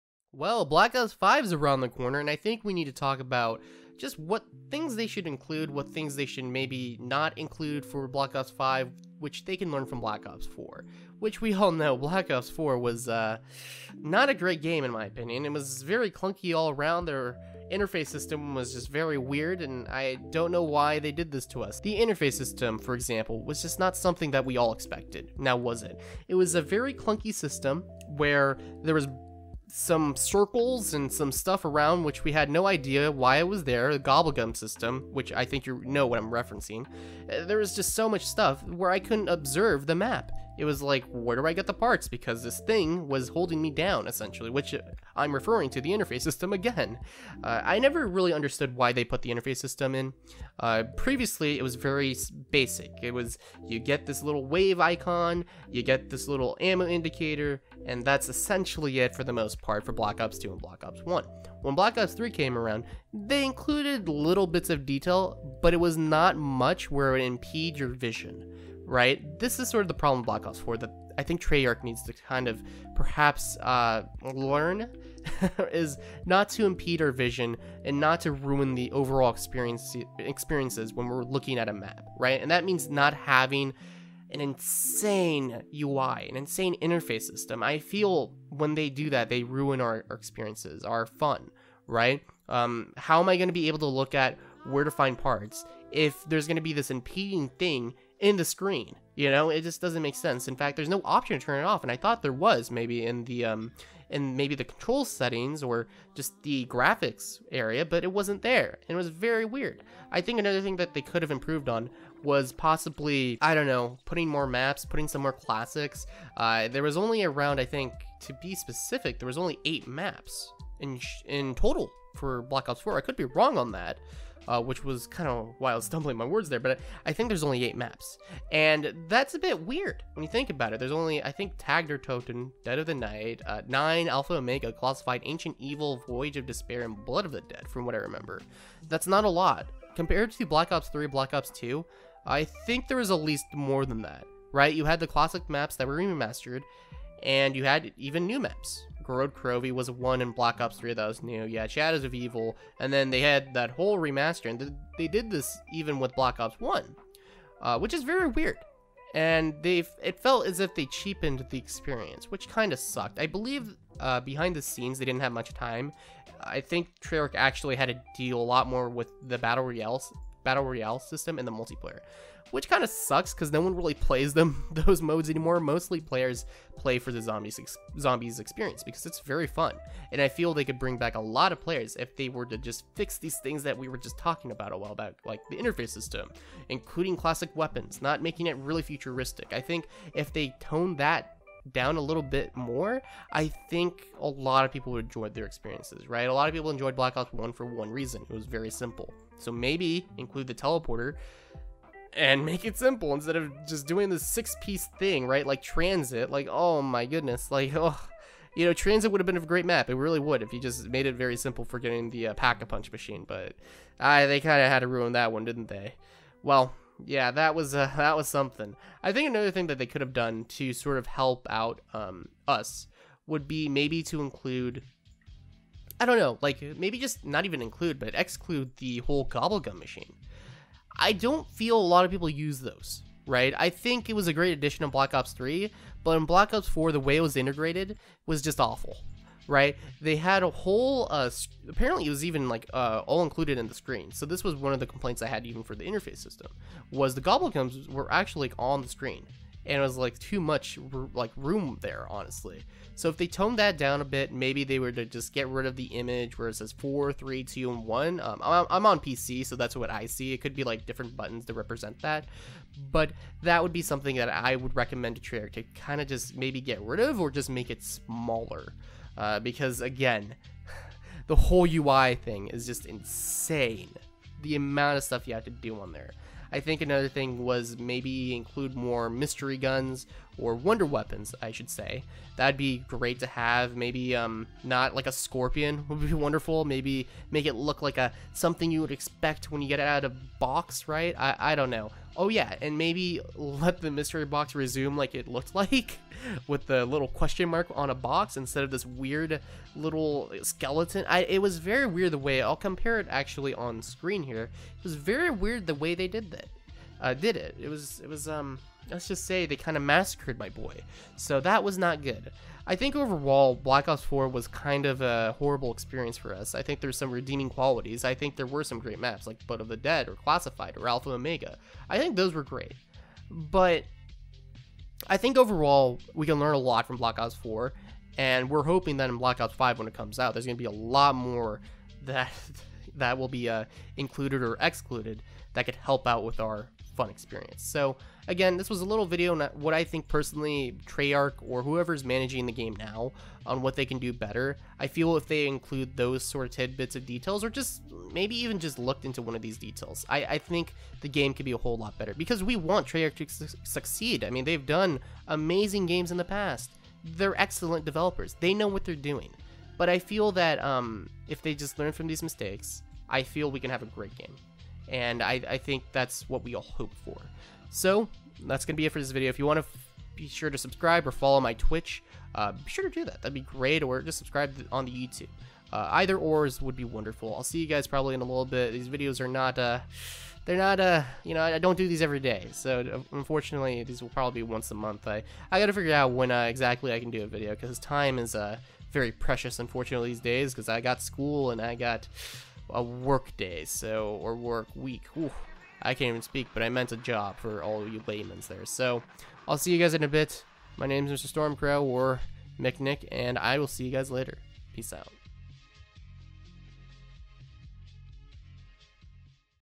What Zombies In Black Ops 5 Needs
What Zombies In Black Ops 5 Needs! Today we talk about what Black ops 5 needs in it's zombies! We talk about the UI interface and what it needs better.